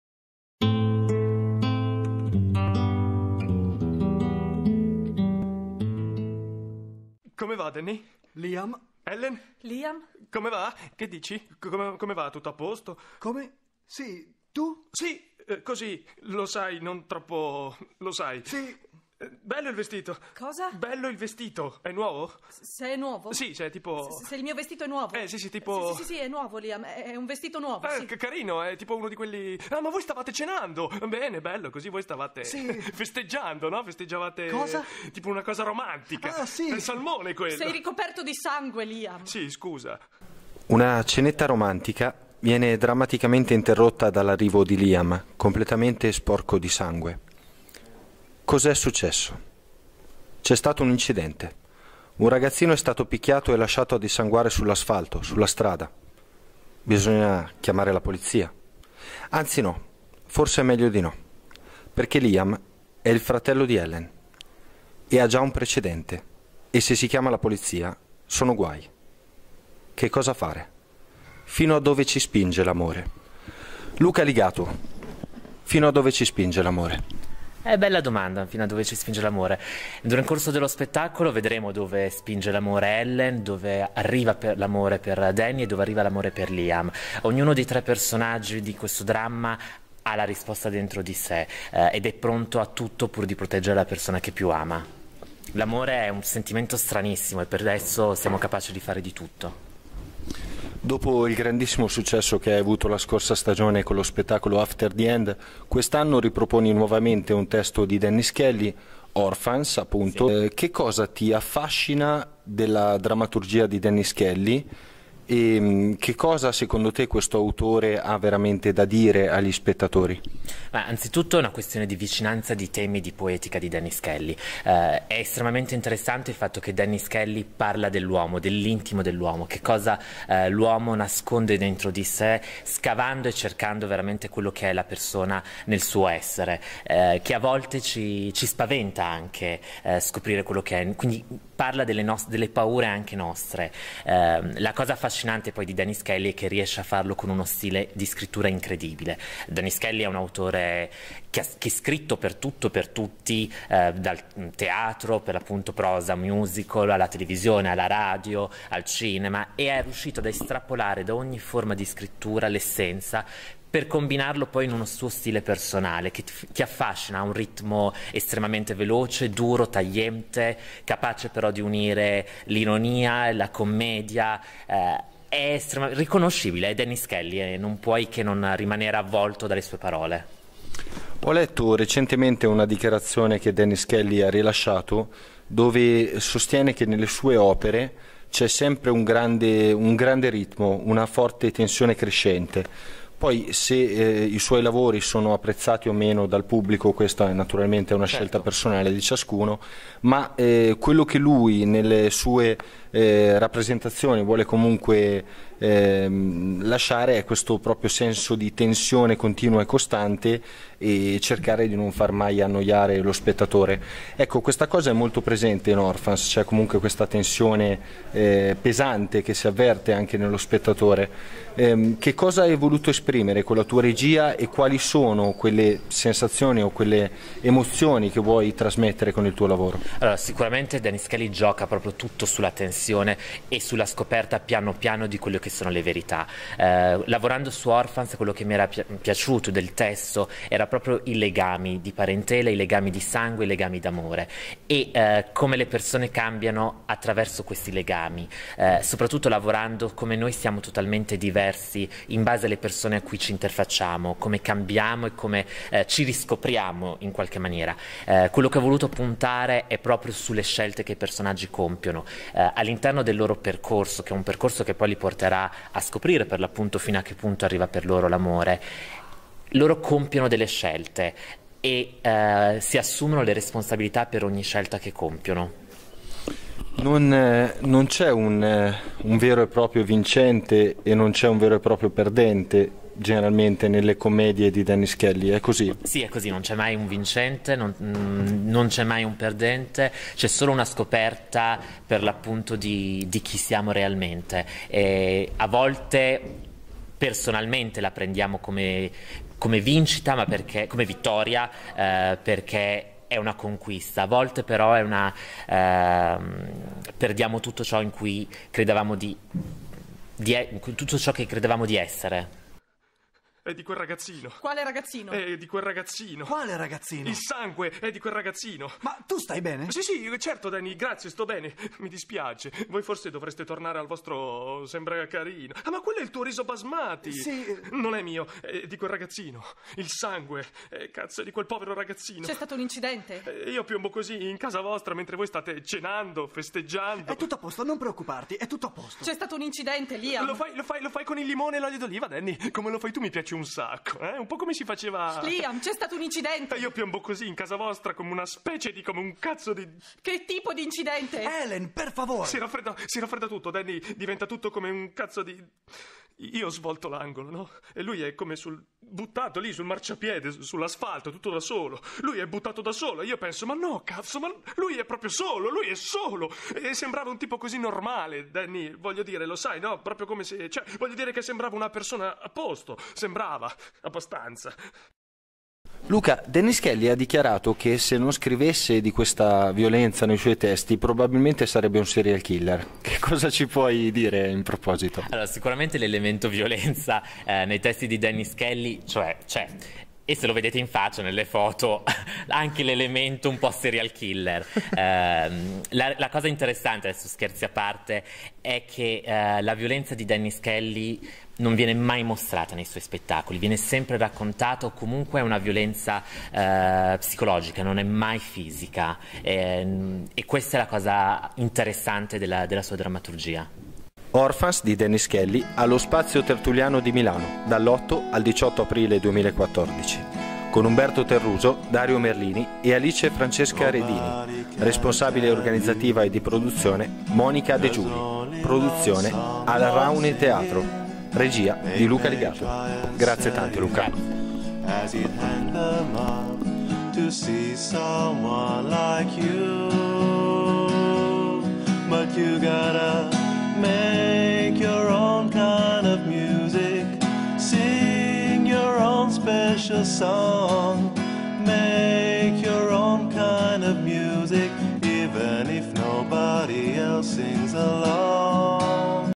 Riccardo Italiano | Conversazione con Luca Ligato
Riccardo Italiano
CONVERSAZIONE CON LUCA LIGATO

In occasione delle repliche di ORPHANS (by Dennis Kelly), Riccardo Italiano incontra il regista Luca Ligato.